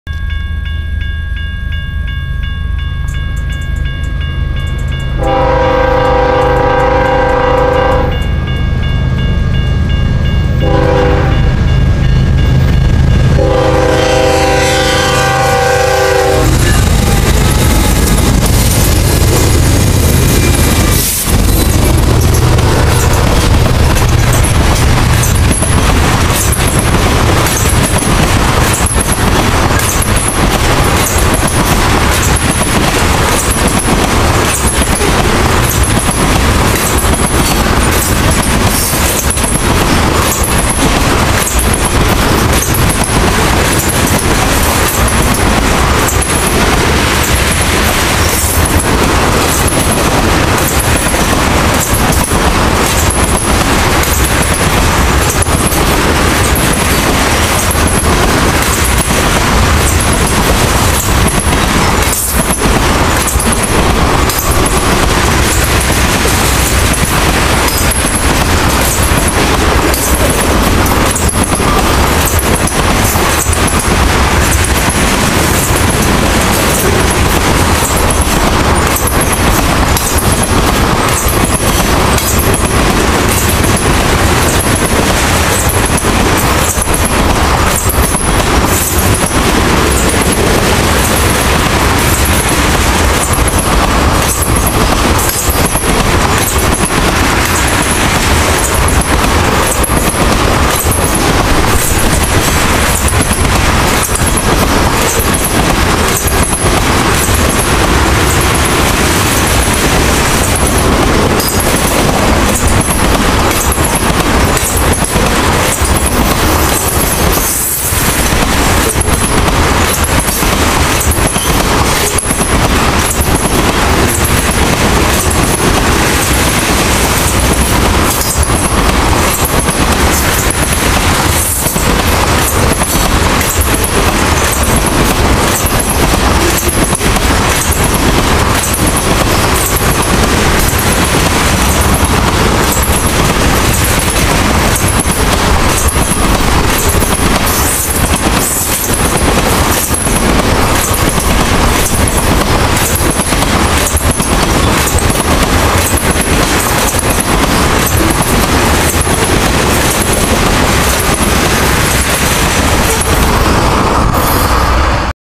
BNSF 7552 Leads Loaded Oil Train
Four BNSF motors pull a loaded oil train EB on the Barstow sub. Filmed in Alpha, IL April 28th, 2010.